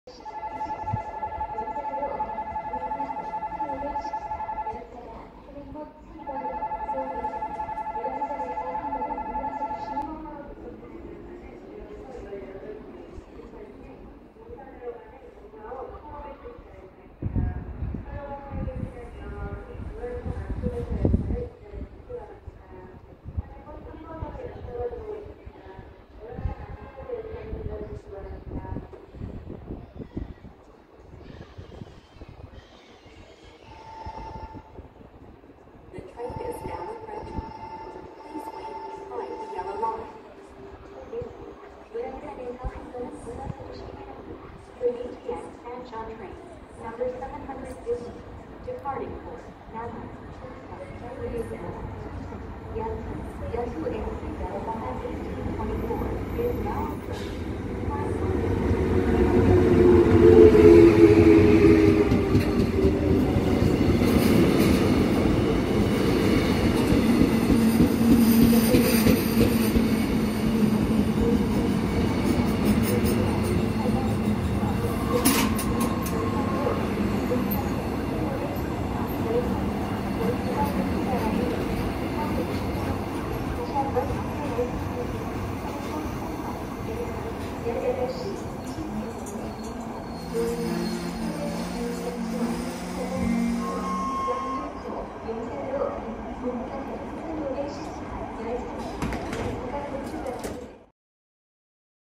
전주역 용산행 무궁화호 #1514 도착(NEL 8227)
2019년 4월 27일 촬영

용산행  무궁화호 열차 전주역 도착

The Mugunghwa train bound for Yongsan, arriving at Jeonju Station